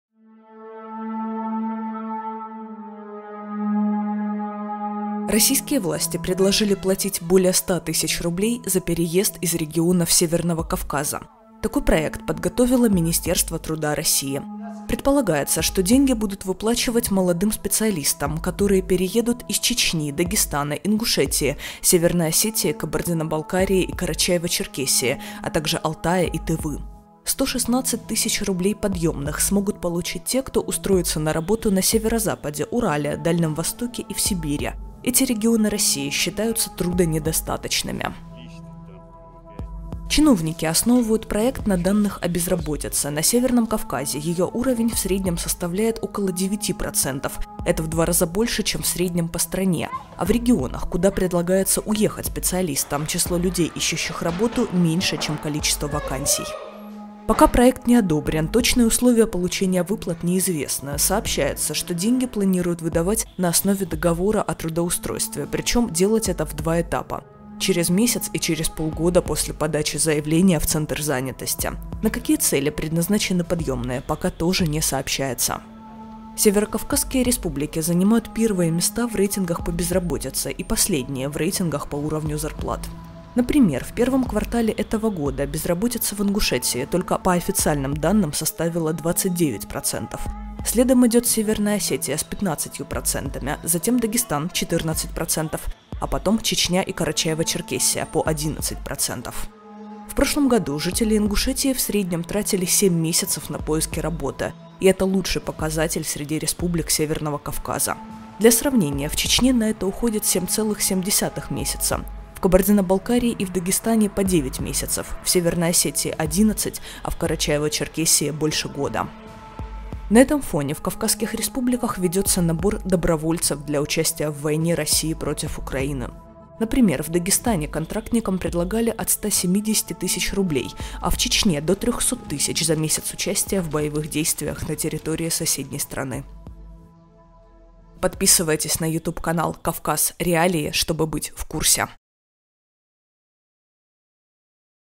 Власти предложили платить за переезд из республик Северного Кавказа | НОВОСТИ
Российские власти предложили платить 116 тыс рублей за переезд из регионов Северного Кавказа. Предполагается, что деньги будут выплачивать специалистам из Чечни, Дагестана, Ингушетии, Северной Осетии, Кабардино-Балкарии и Карачаево-Черкесии (а также Алтая и Тывы).